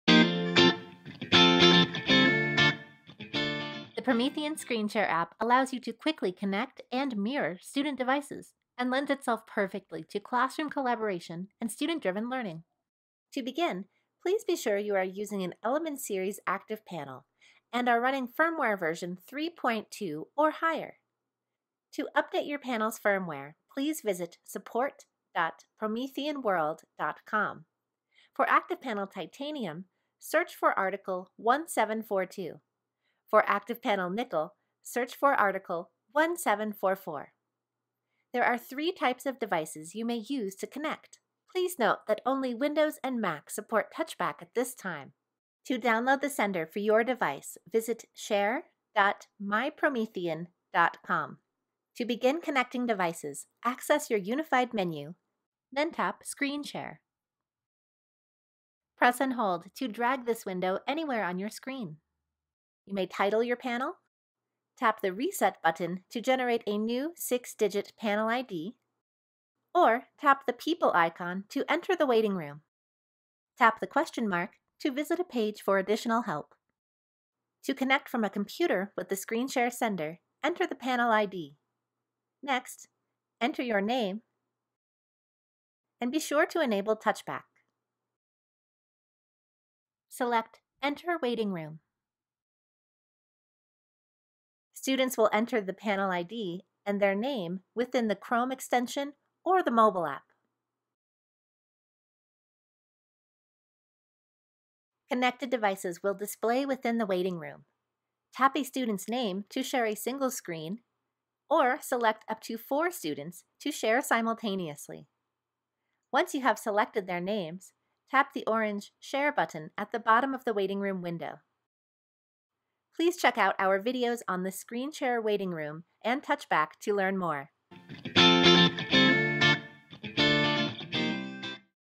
Screen Share: Connecting Devices
Learn how to Connect Devices to ScreenShare on your Titanium and Nickel ActivPanels.